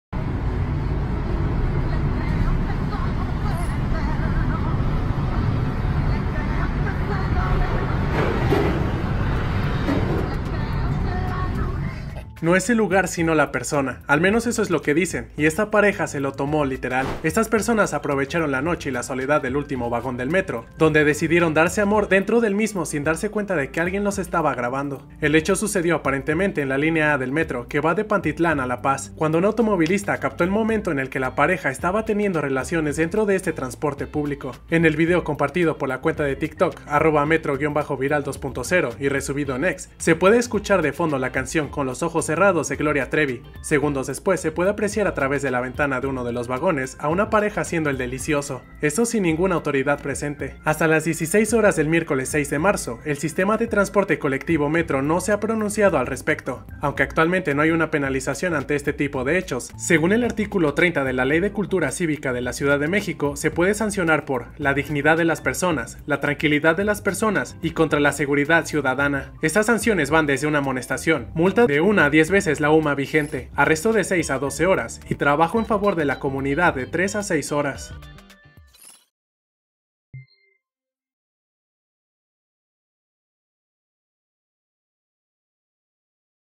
Grabados en el acto: Cachan a pareja dándose amor en el último vagón del metro
Dicen que “No es el la persona sino el lugar” y por ello dos hombres aprovecharon la soledad del último vagón del metro y la noche para hacer el “delicioso”, esto sin saber que los estaban grabando